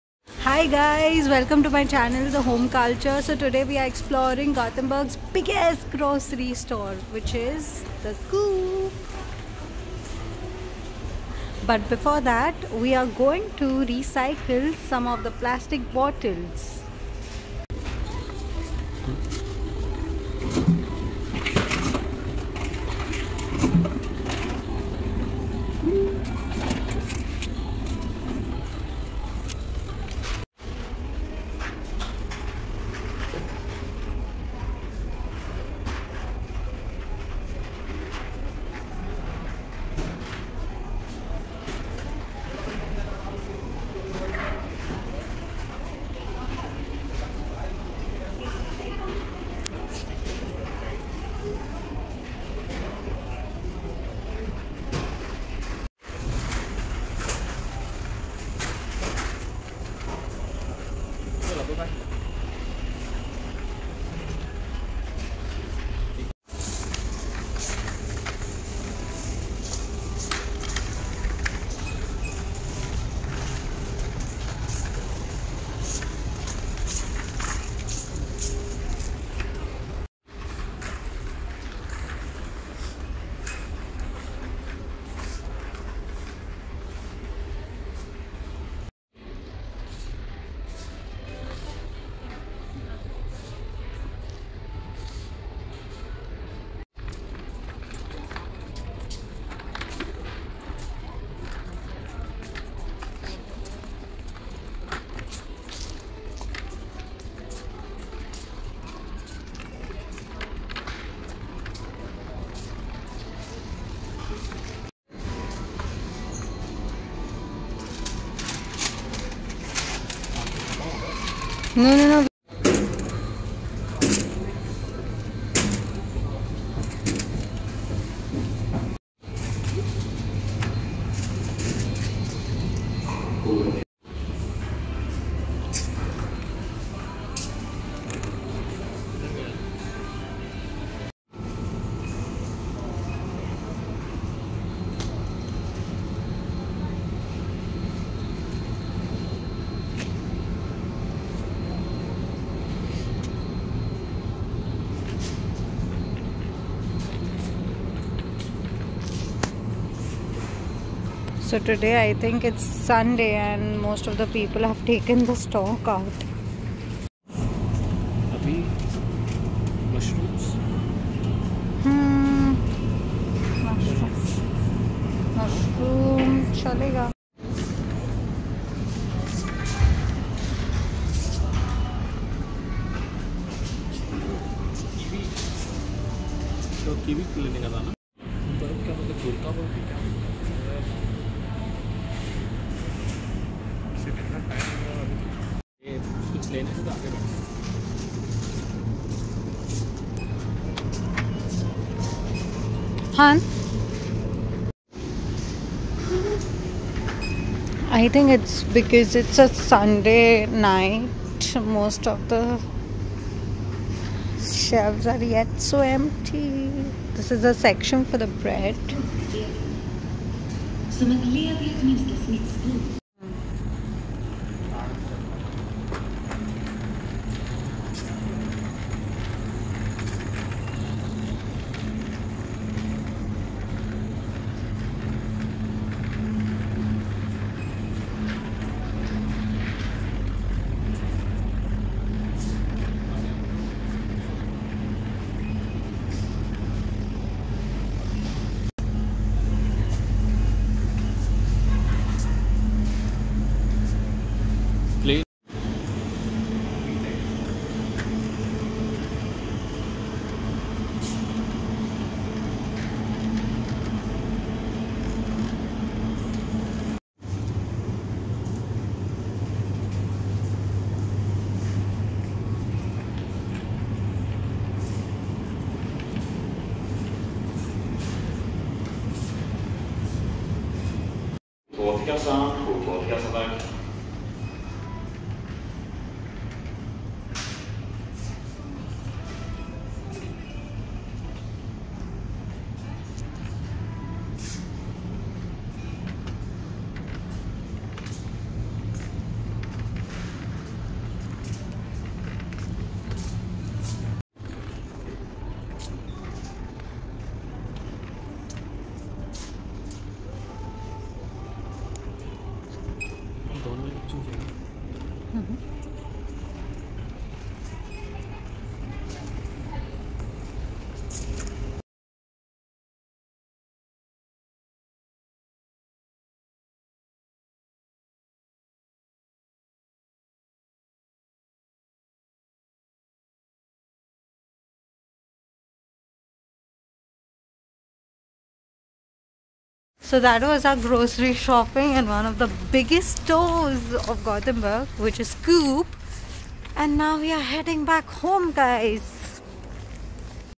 2021 l COOP - Biggest store for Grocery shopping
Me and my husband often go for grocery shopping together. Preparing a list of seasonal food to get - It's fun exploring the store. We went to the biggest stores in Gothenburg for shopping it's named 'COOP'.